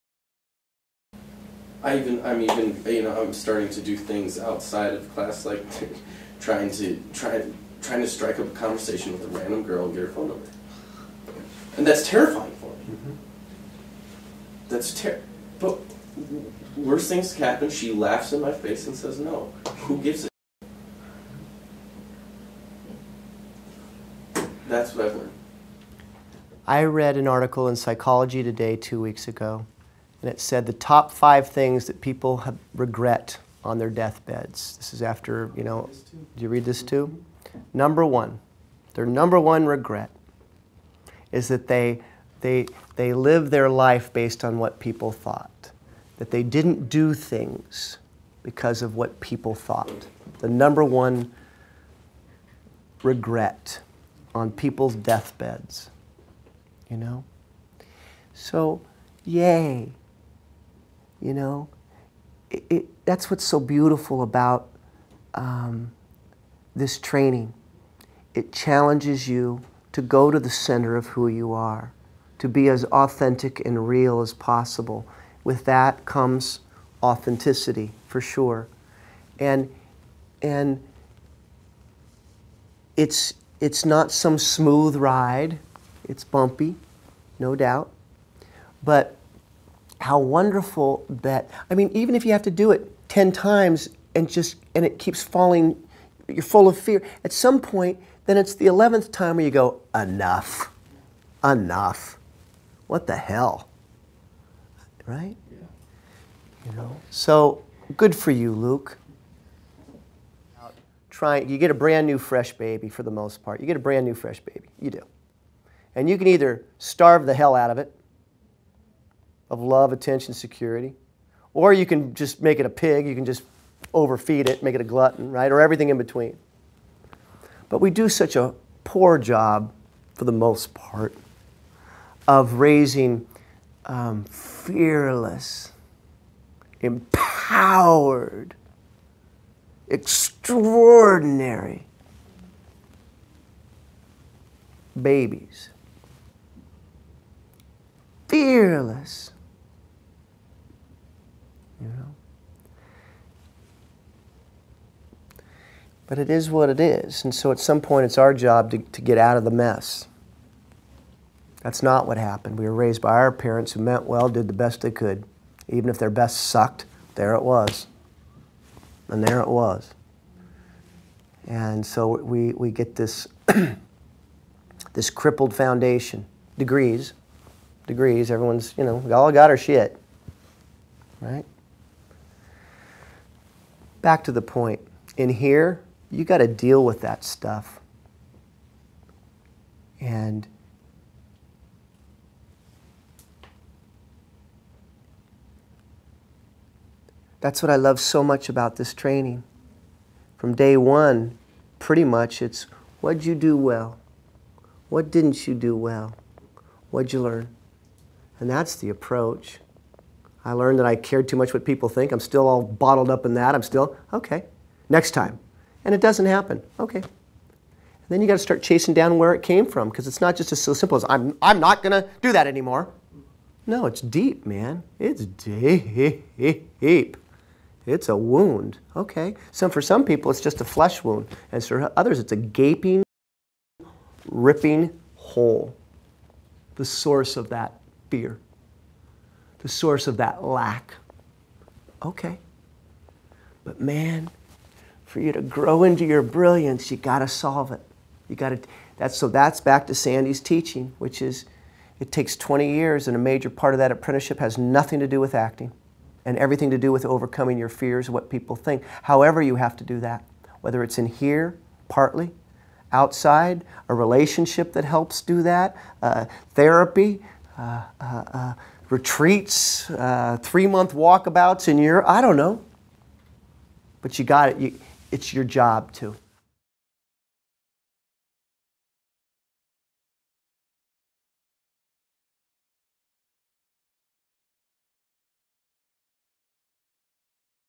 Why Study the Meisner Technique
In this teaching, Jim Jarrett talks about the incredible gifts that The Meisner Technique will bring to your acting and just as importantly, to your life. We hope you enjoy!